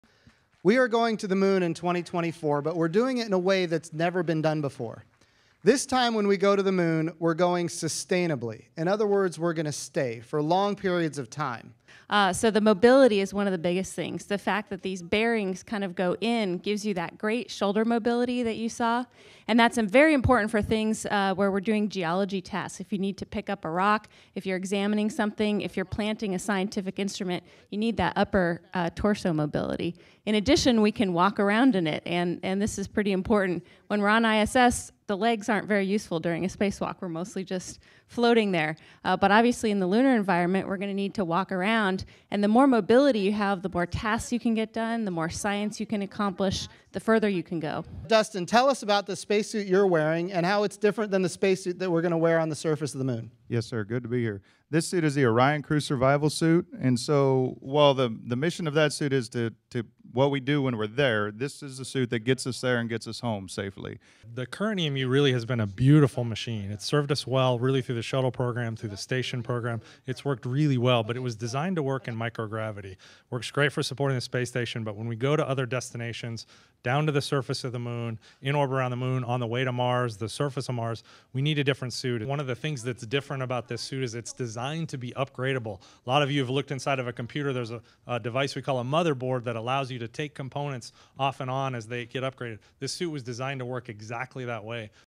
NASA unveils new spacesuits for Artemis program
NASA has unveiled two next generation spacesuits for its Artemis program as the space agency prepares to send astronauts to the moon by 2024. (Oct. 15)